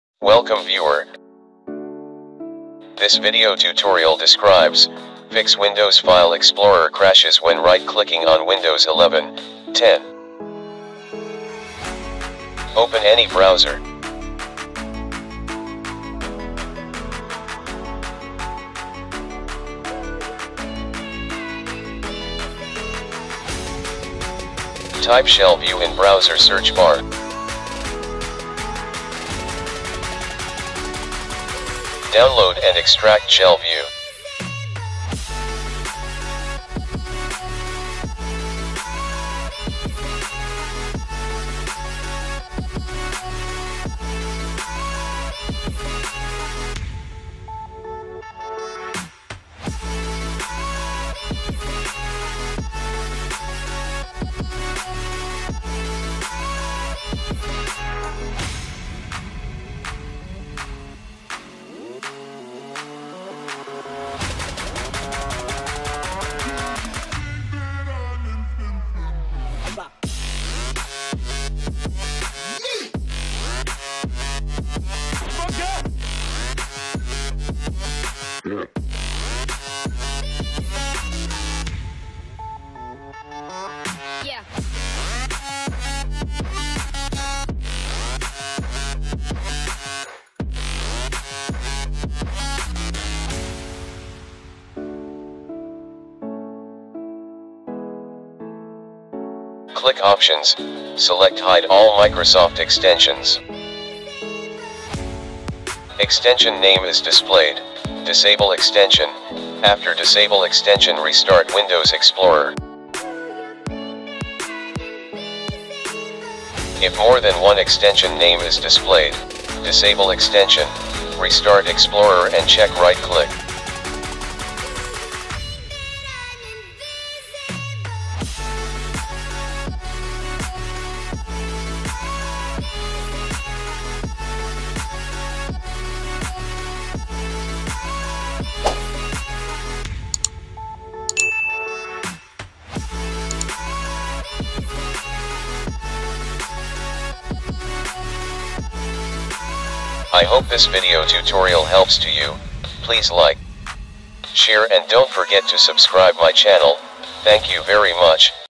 Windows File Explorer Crashes When Right Clicking On Windows 11/10
This video helps you to Fix Windows File Explorer Crashes When Right Clicking On Windows 11/10

_________________Hash Tags_________________